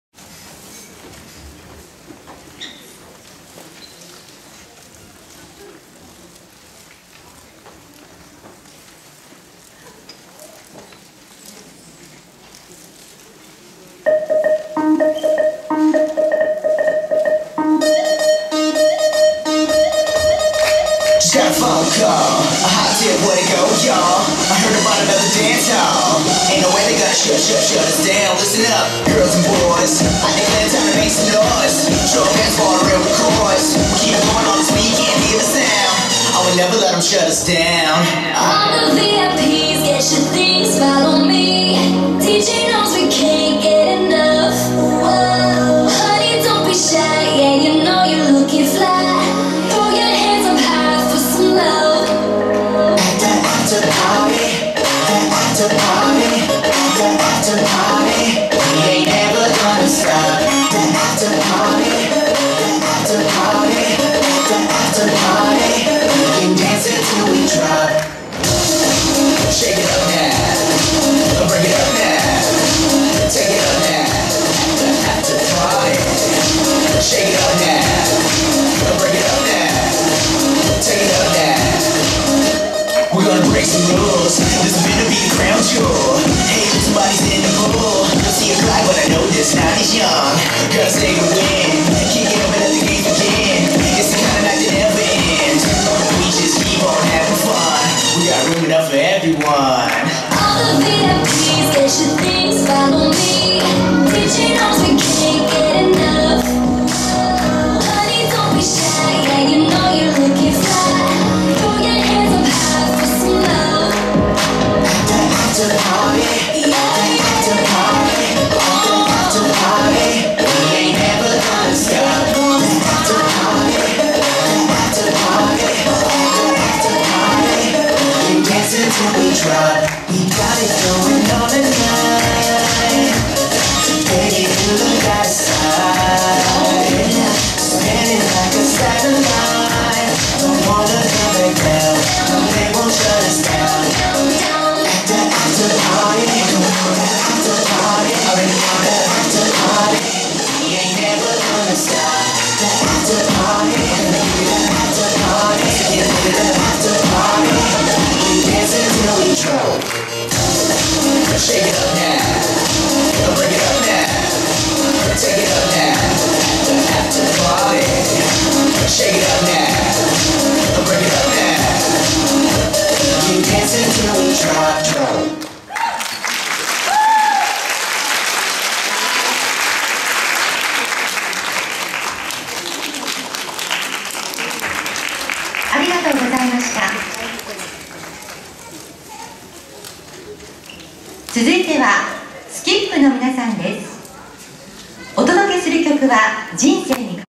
【ダンス作品】「After Party」（DANCE for REAL）☆（「第４回チャリティ・HEARTステージ」（2014年3月2日））
「第４回チャリティ・HEARTステージ」（2014年3月2日）で発表したダンス作品「After Party」本番。「DANCE for REALキッズ&Jr親子クラス」で踊りました♪